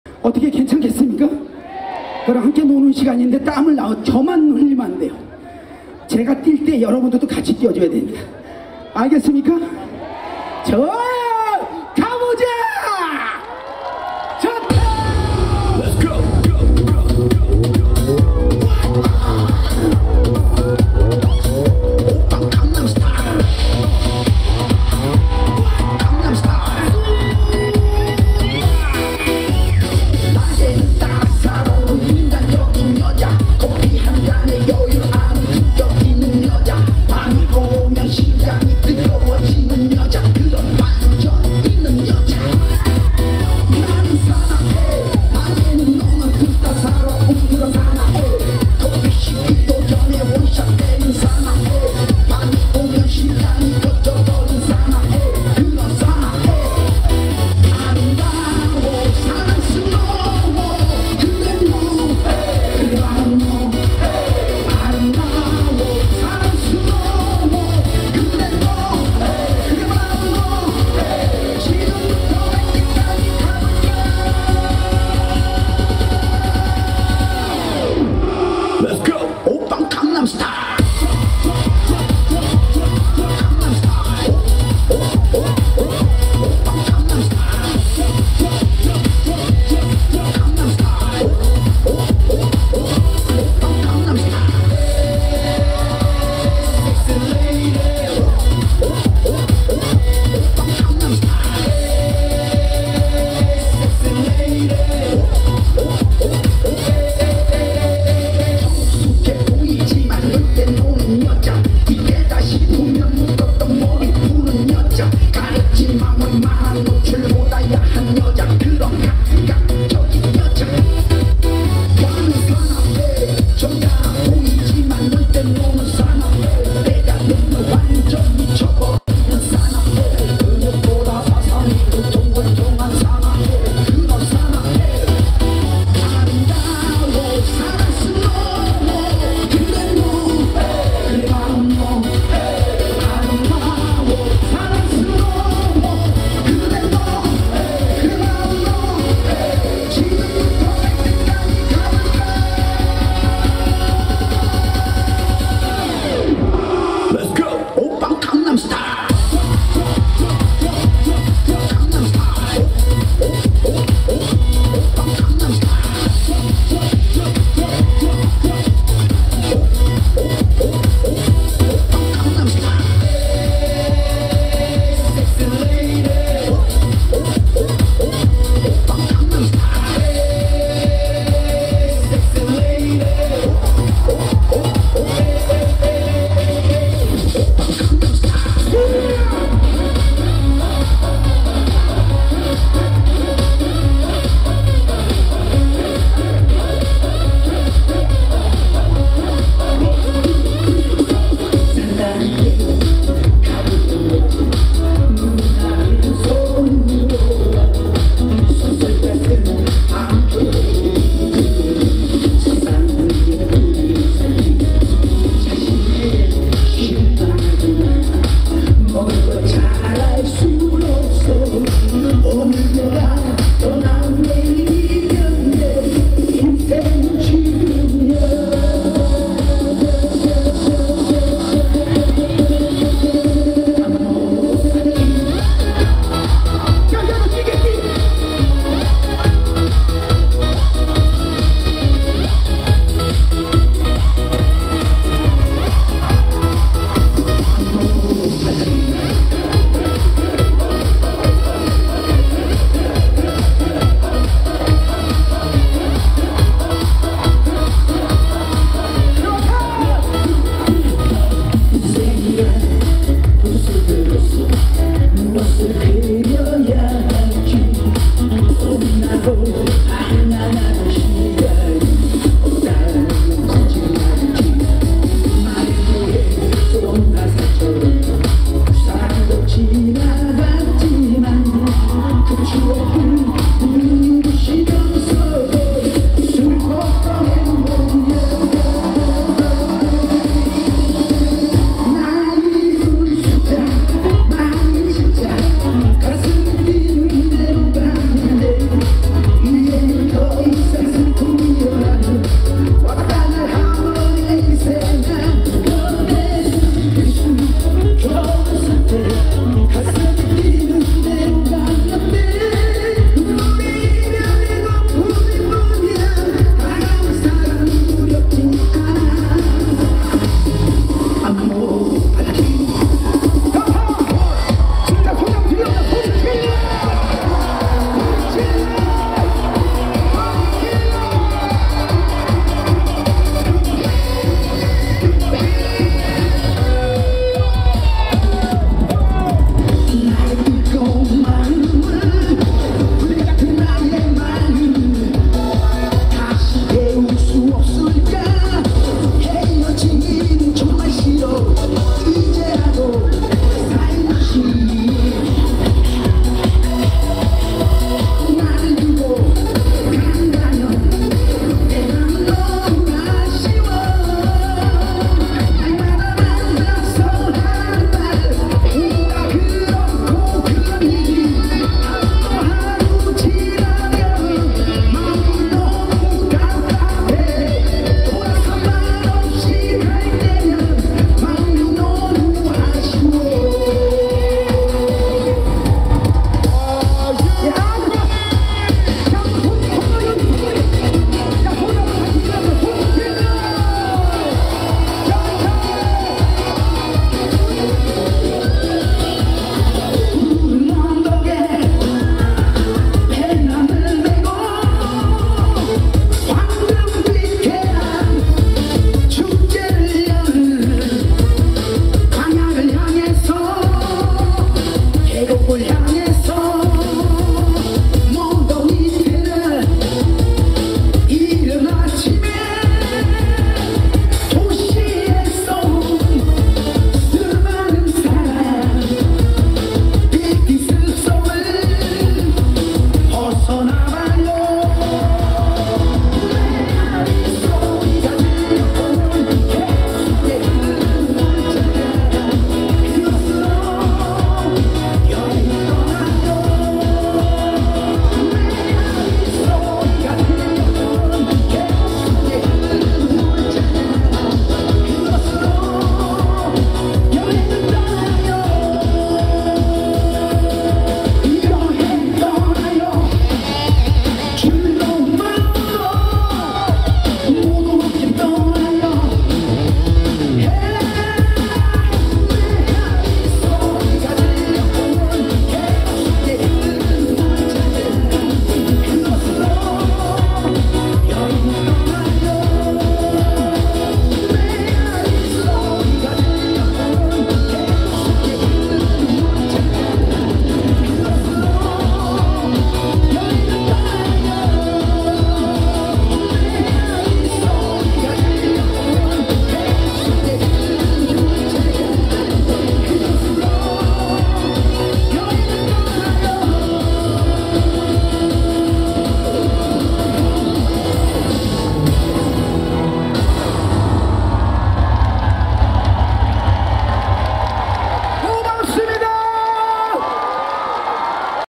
2019봉화 은어축제 초대가수 이병철 공연 2019 -07- 27
2019봉화 은어축제 초대가수 이병철
장소,봉화 내성천 일원